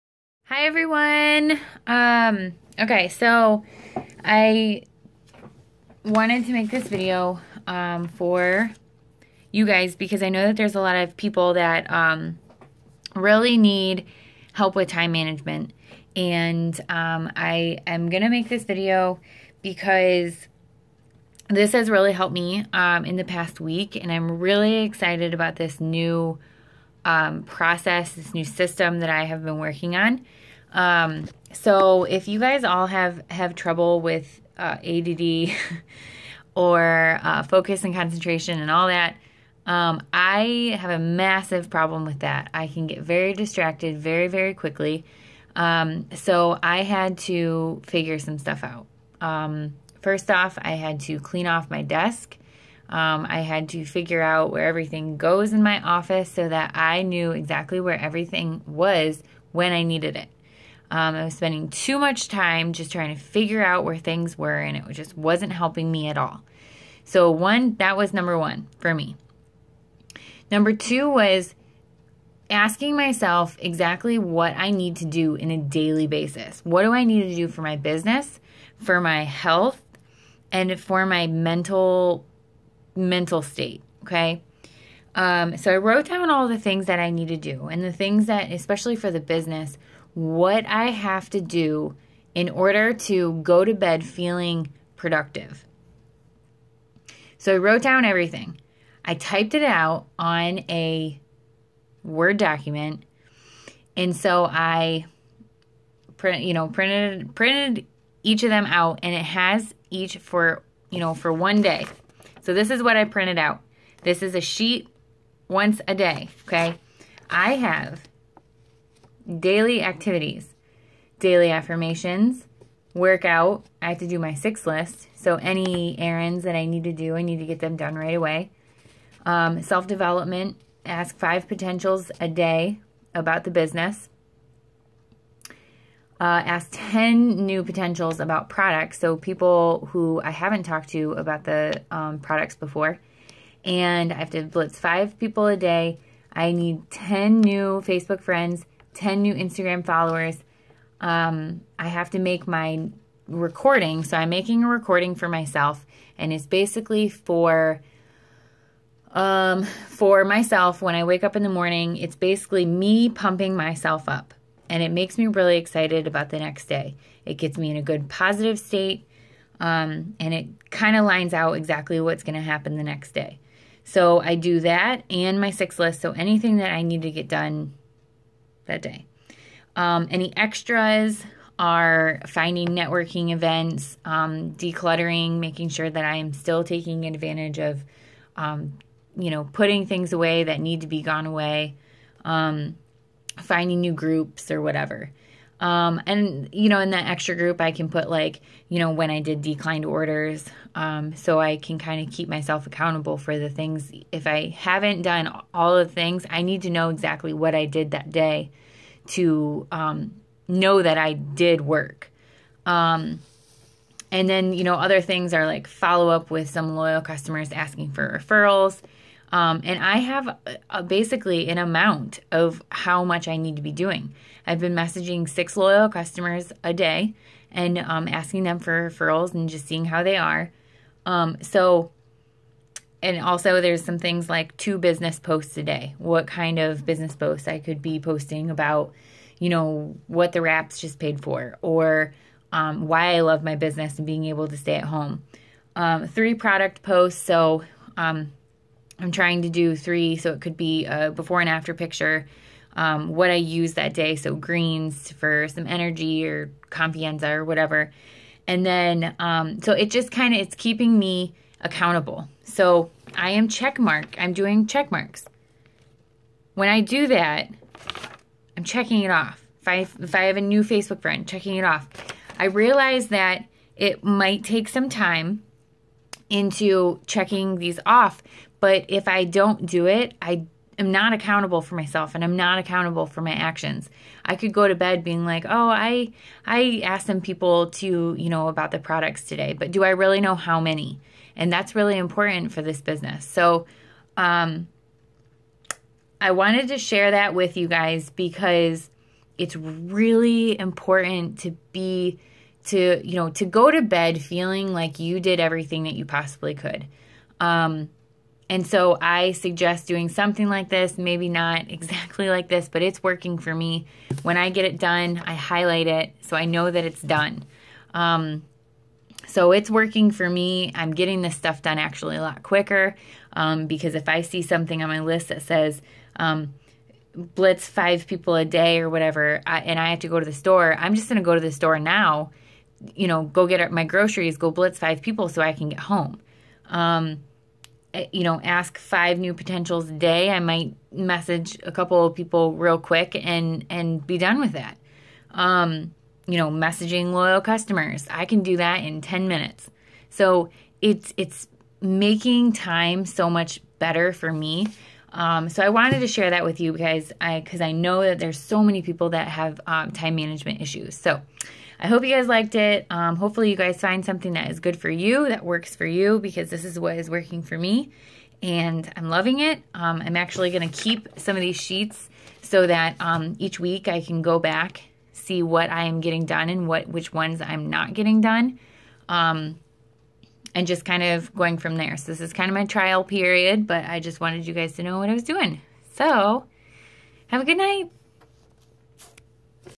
Time Management Tips
Tackling your to do list and figuring out what you MUST do every day!